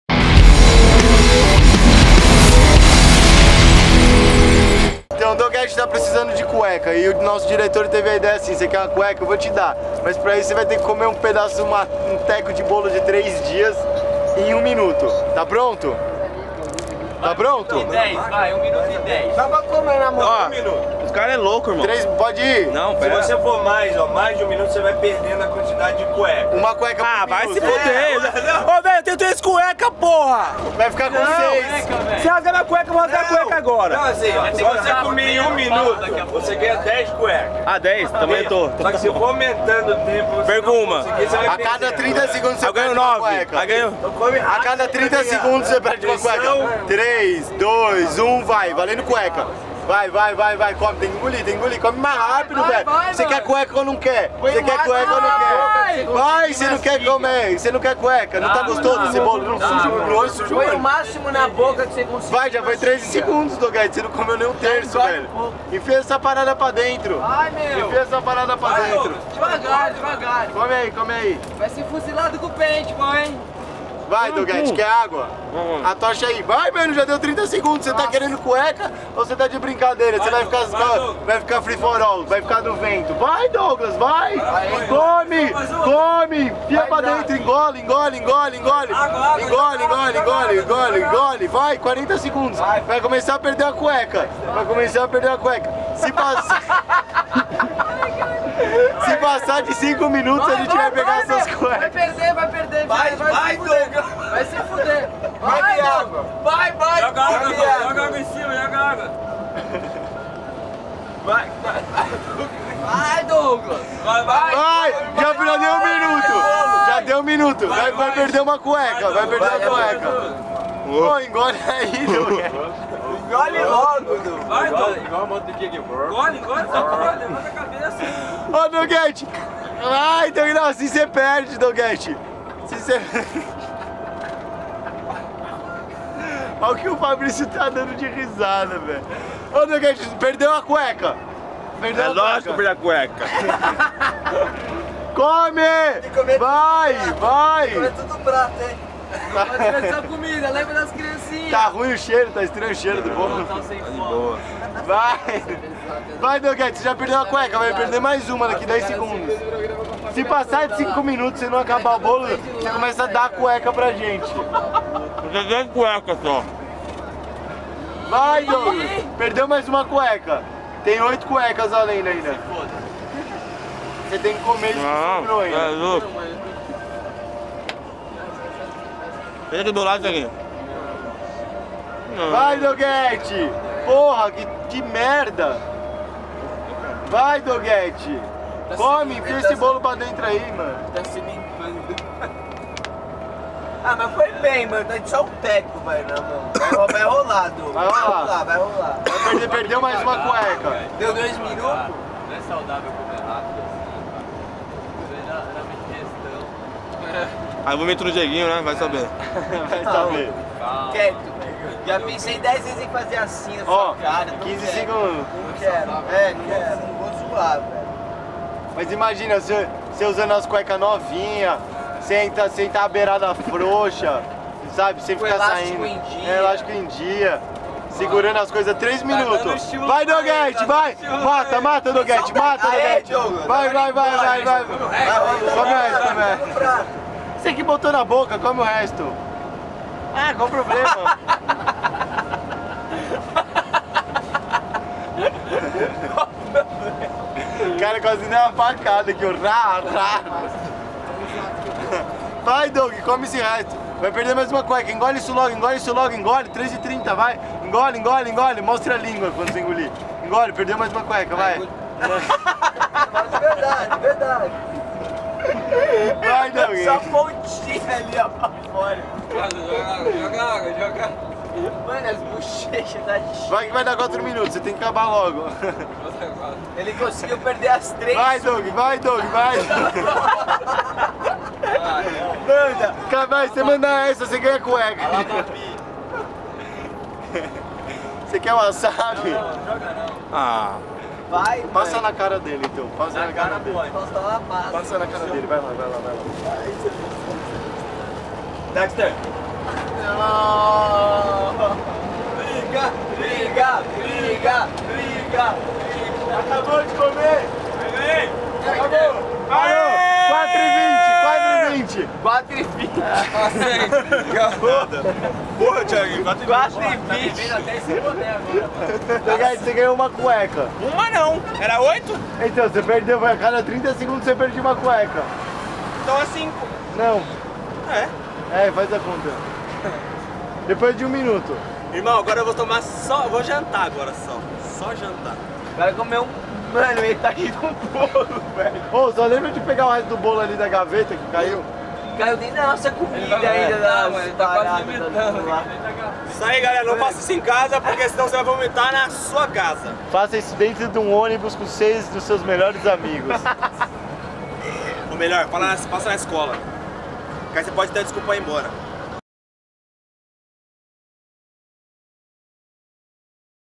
Deco e Lucas na Rota Explosiva - Cena Inédita - Bolo por cuecas novas
O menino Douglas precisava de novas cuecas. A gente teve uma idéia.

YouTube.com/user/BolovoProductions